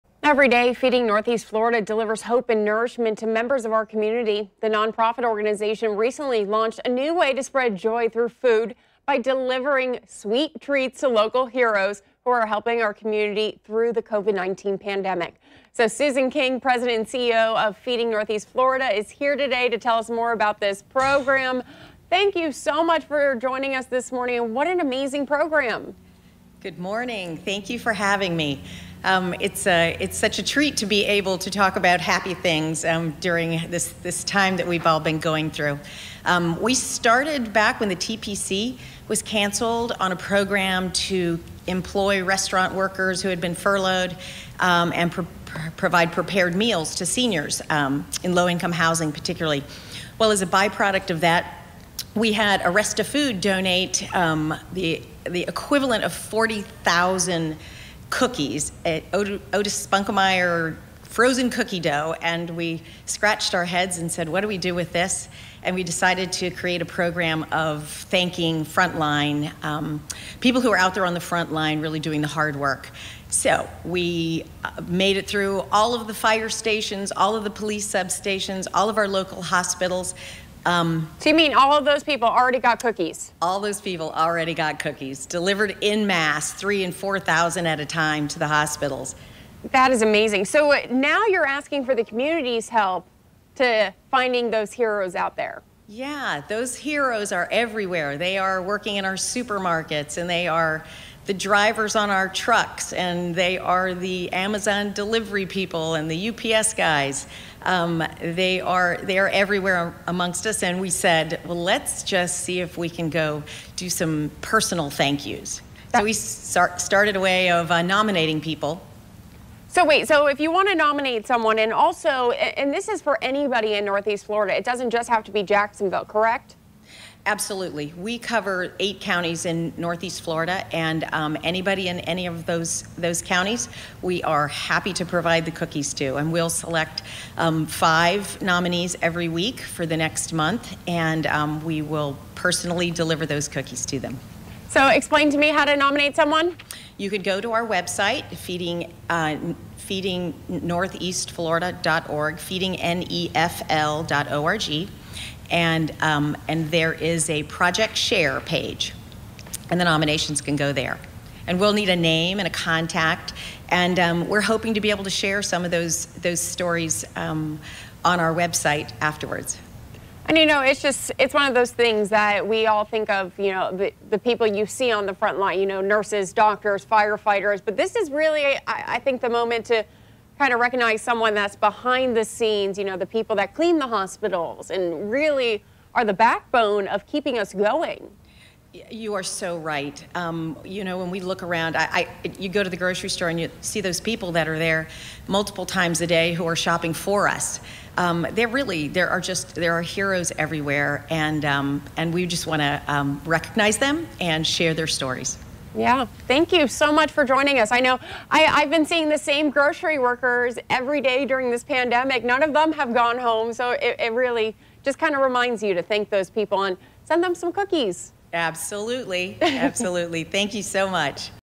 A local program is delivering sweet treats to local heroes
Susan King, President and CEO of Feeding Northeast Florida, is here today to tell us more about this program, and how you can nominate people in your life to receive a special delivery.